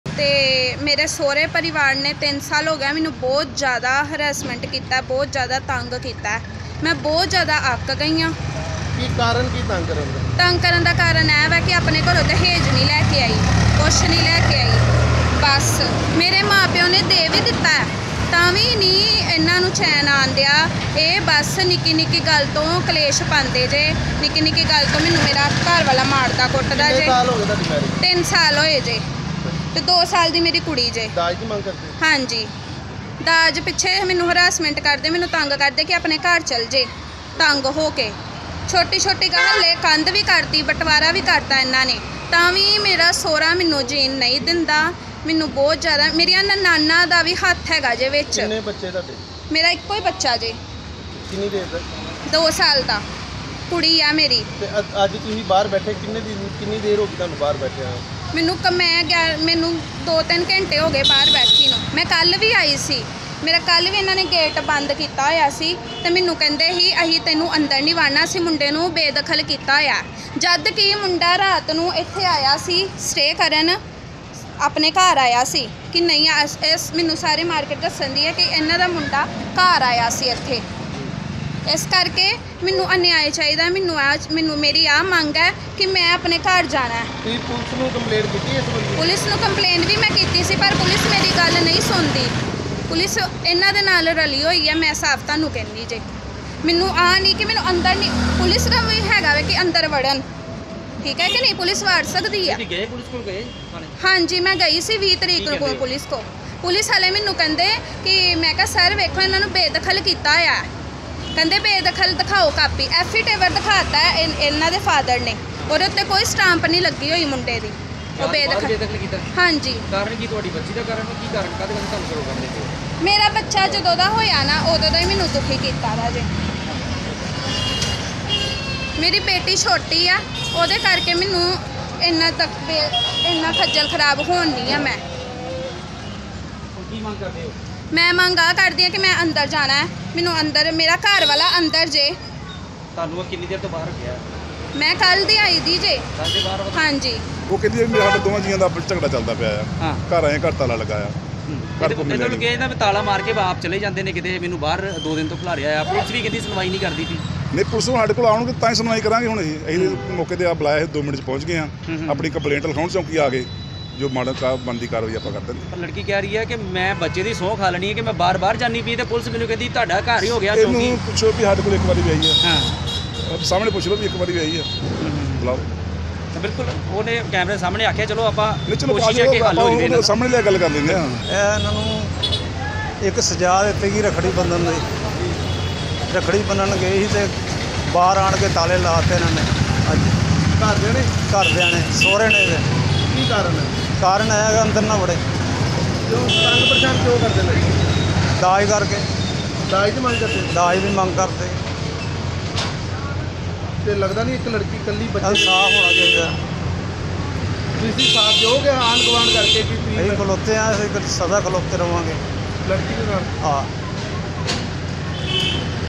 Amritsar News | ਨੂੰਹ ਨੇ ਕੀਤਾ ਸਹੁਰੇ ਘਰ ਬਾਹਰ ਹਾਈਵੋਲਟੇਜ ਡਰਾਮਾ | Hungama | Punjab Police | N18V
Amritsar News | ਨੂੰਹ ਨੇ ਕੀਤਾ ਸਹੁਰੇ ਘਰ ਬਾਹਰ ਹਾਈਵੋਲਟੇਜ ਡਰਾਮਾ,  ਘਰ ’ਚ ਐਂਟਰੀ ਨਹੀਂ ਦੇ ਰਹੇ ਸਹੁਰੇ | N18V